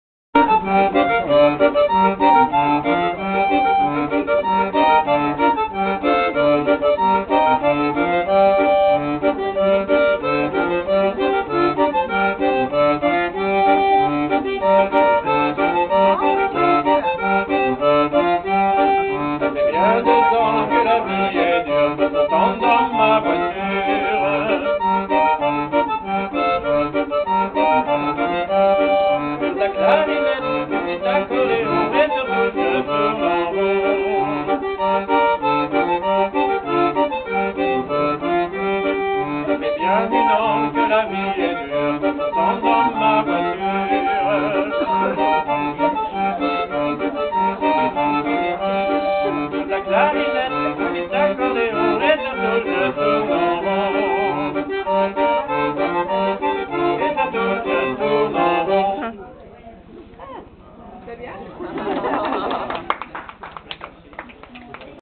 Yves Leblanc - Orgue de Barbarie - Rond de Saint Vincent.mov
Yves Leblanc playing his 'barrel organ' at the Festival Des Panards in Otley, Yorkshire UK in October 2010.  The tune is a Rond de Saint Vincent.  Later in the evening, Yves played and sang a selection of Breton dance tunes and songs on both his barrel organ and his melodeon which we really enjoyed dancing to!